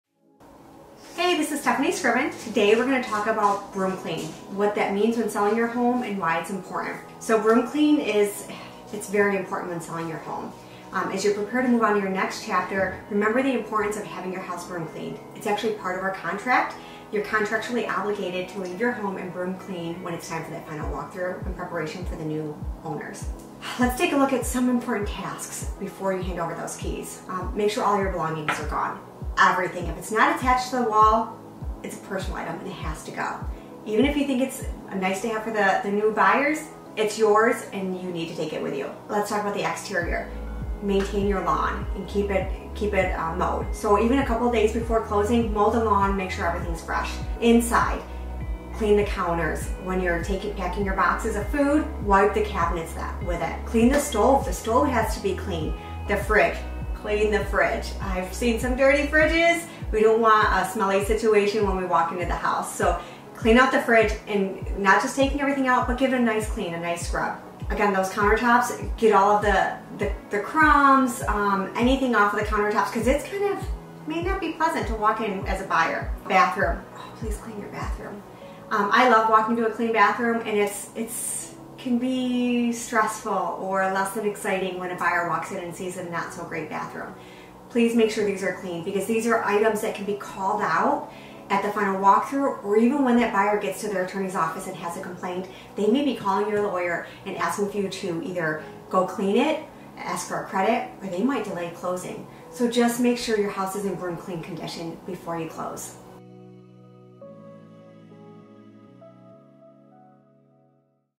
Broom Clean
Broom clean is an essential part of the purchase contract. A clean home simplifies the closing process. It minimizes the chances of last-minute disputes or delays related to cleanliness issues. A clean home can expedite the closing, saving both parties time and stress.